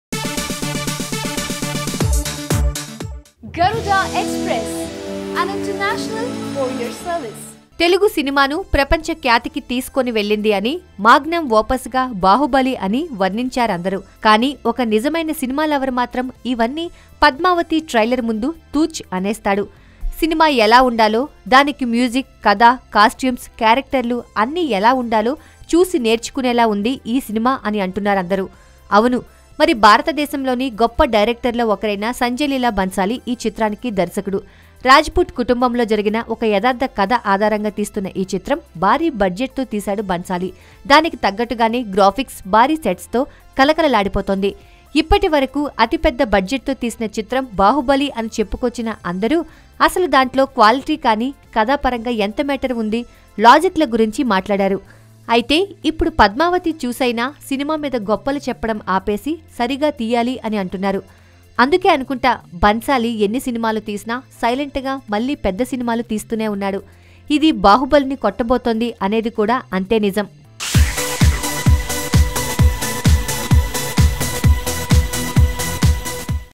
బాహుబలిని మించిన సినిమా || Padmavati Theatrical Trailer Effect On Baahubali || YOYO Cine Talkies
Padmavati Theatrical Trailer Effect On SS Rajamouli Baahubali
బాహుబలిని మించిన సినిమా.